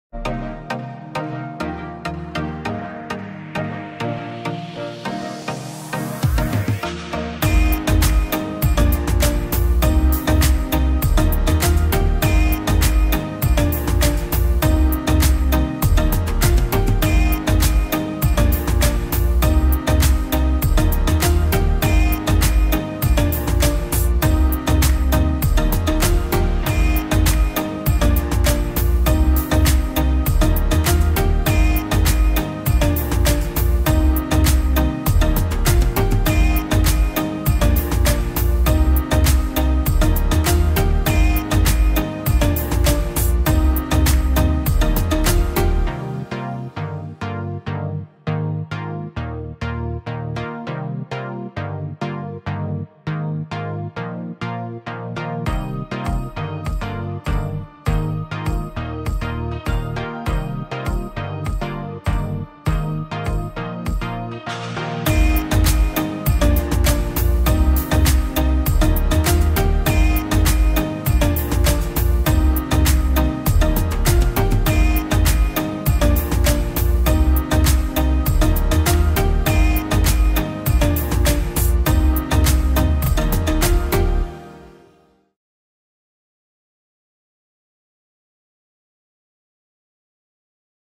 Technomelt Supra 081 ECO. The world's first carbon negative hot melt
Start saving Carbon immediately by switching to Technomelt Supra 081 ECO